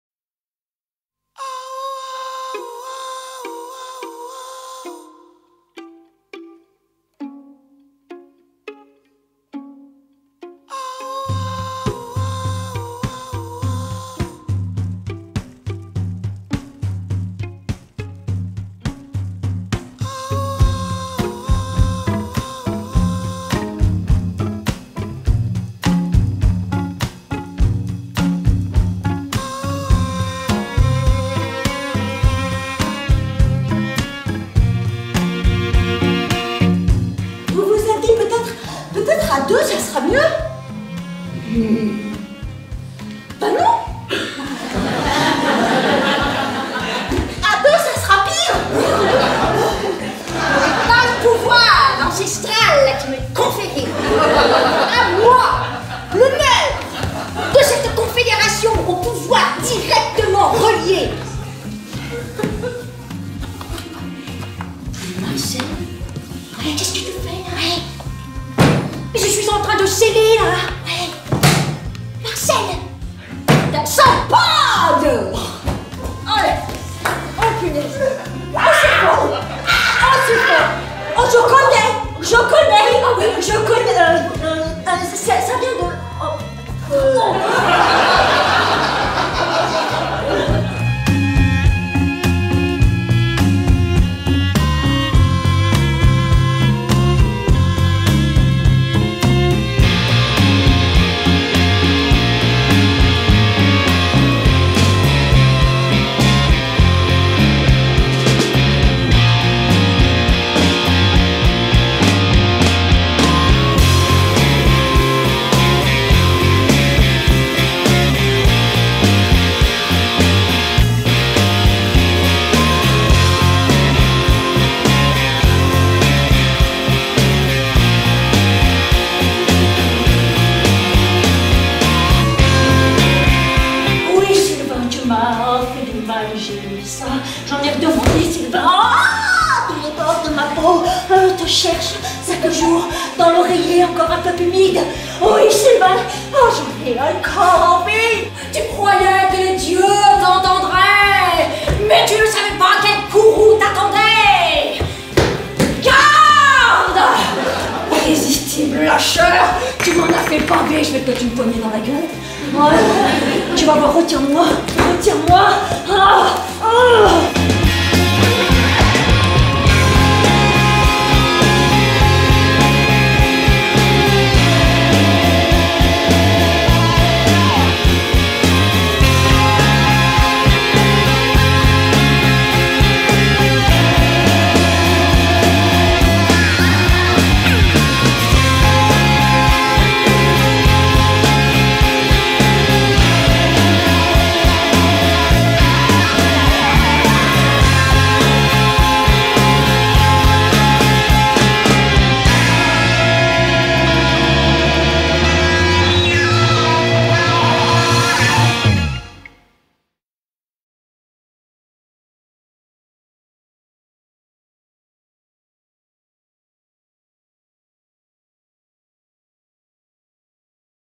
Serial Tulleuses - Cie Canon
"Rita", "Marcelle" et "Urga" sont les demoiselles d'honneur d'un mariage qu'elles vont dézinguer... Les avoir comme invitées s'avère dangereux !

Au fur et à mesure que le spectacle se déroule, la bienséance dérape vers des comportements plus pulsionnels. Du rire aux larmes, il n'y a qu'un souffle. Sur la corde sensible avec toujours une bonne dose de dérision, elles dévoilent leur être profond et mettent à nue  leur part sauvage de féminité...

De et par Chloé Bourgois, Karine Larivière et Hélène Risterucci
Collaboration artistique : Erwan David
Création lumière : Fabrice Peineau

Captation réalisée à l'école-théâtre LE SAMOVAR dans le cadre de la 12ème édition du Festival des clowns, burlesques et excentriques